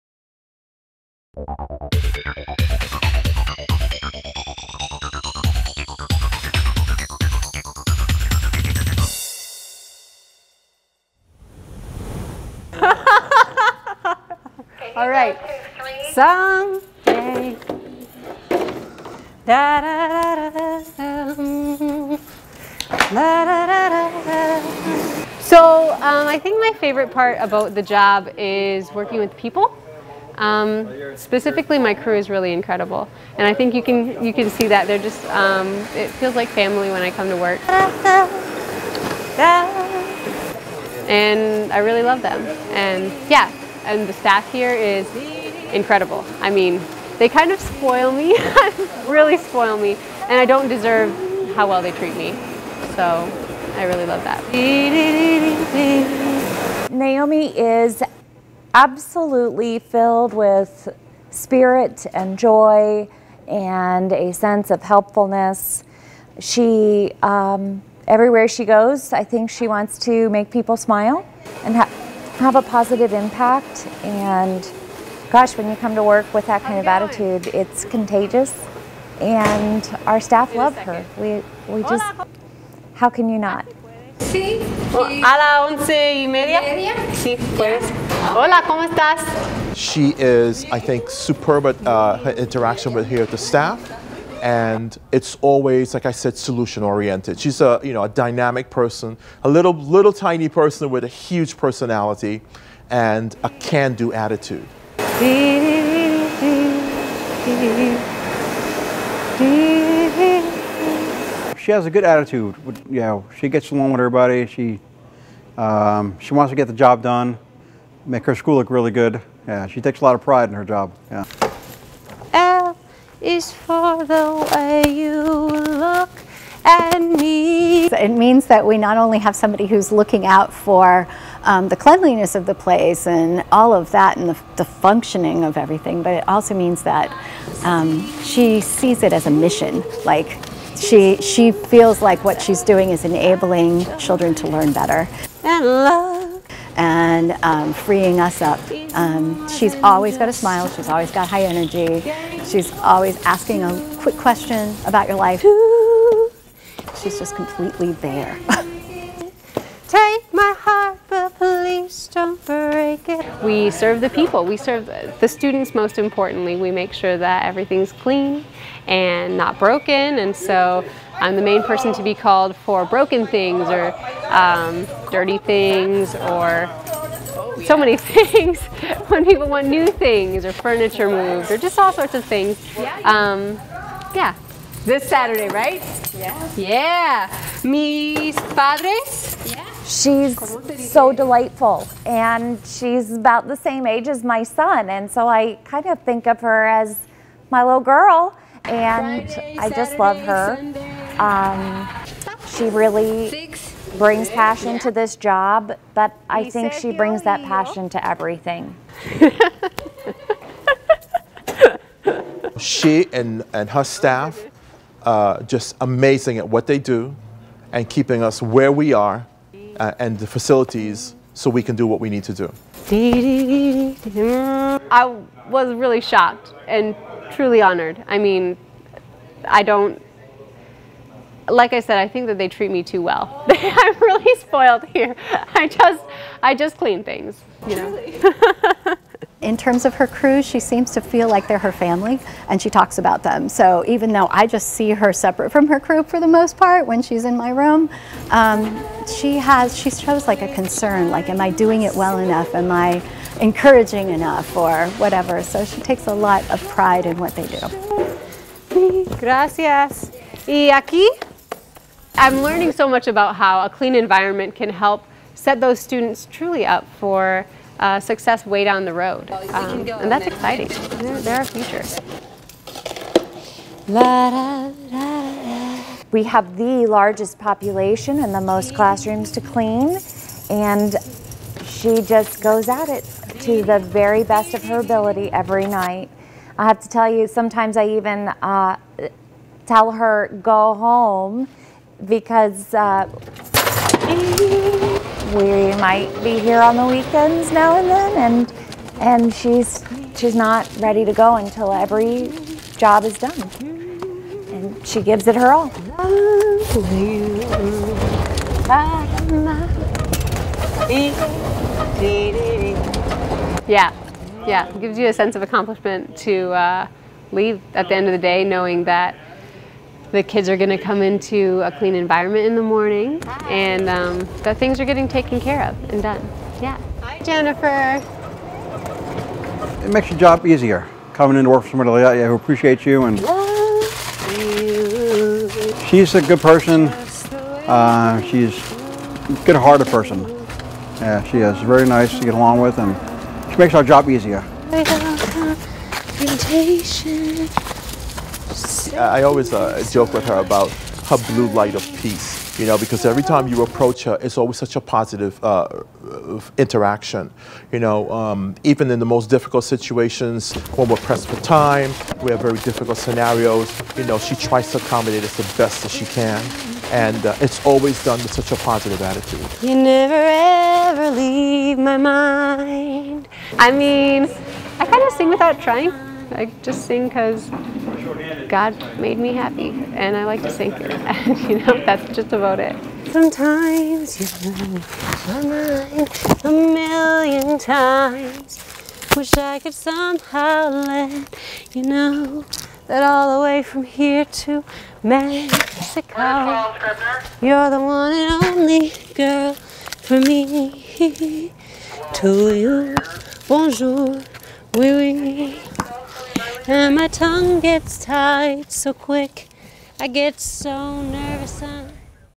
Education Spotlight-Sarasota Middle School-Employee of the Year
Naomi Yoder is the Head Custodian, and has a song in her heart, and a song for when she works.  Naomi is the schools employee of the year.  Producer Larry Phillips caught up with Naomi as she sang away as she & her crew worked away, and found out why she loves her work, and why it's important to her.